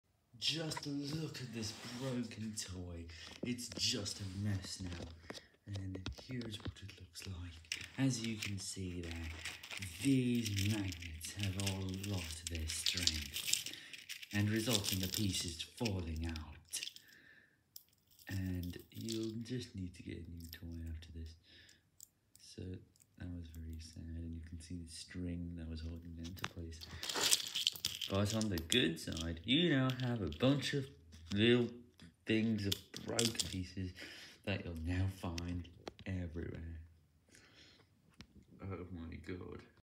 Look at this broken toy.
I wanted to post this video to YouTube after the toy suddenly broke. You can also see the string holding it all.